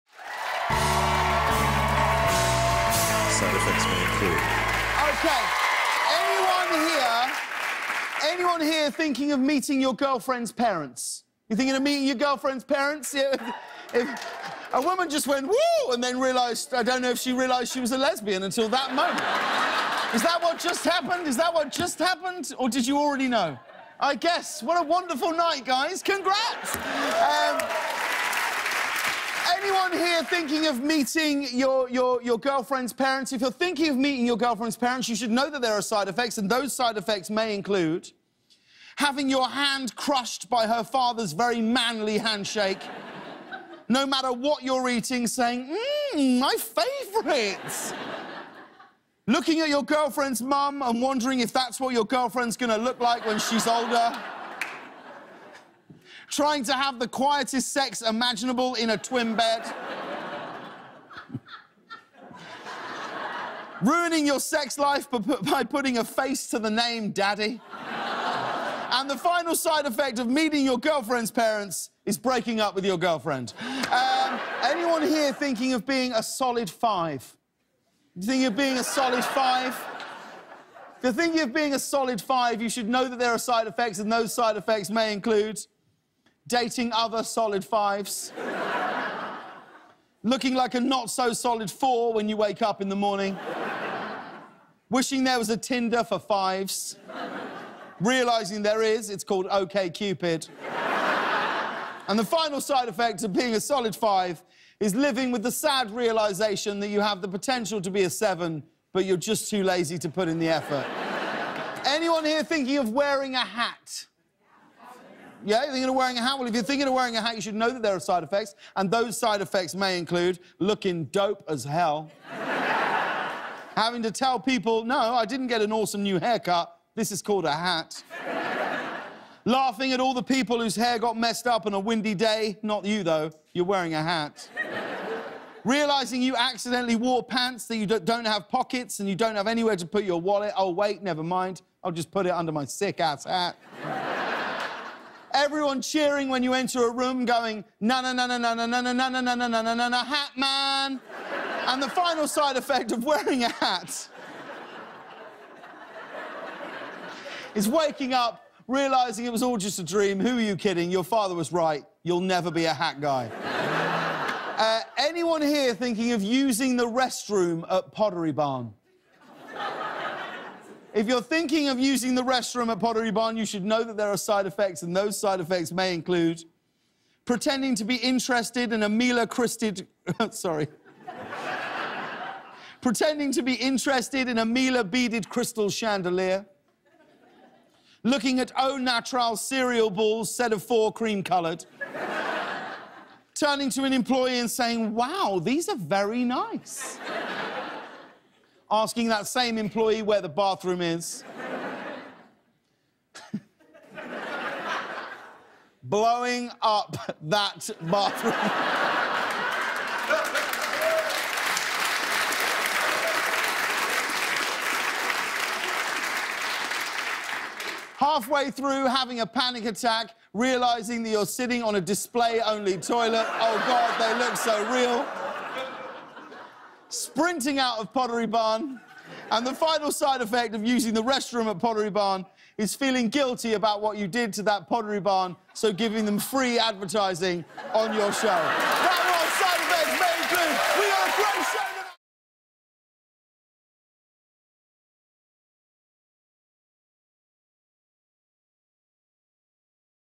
Side Effects May Include: Hats, Pottery Barn Bathrooms
James shares the hidden side effects of things you do and people you meet every day.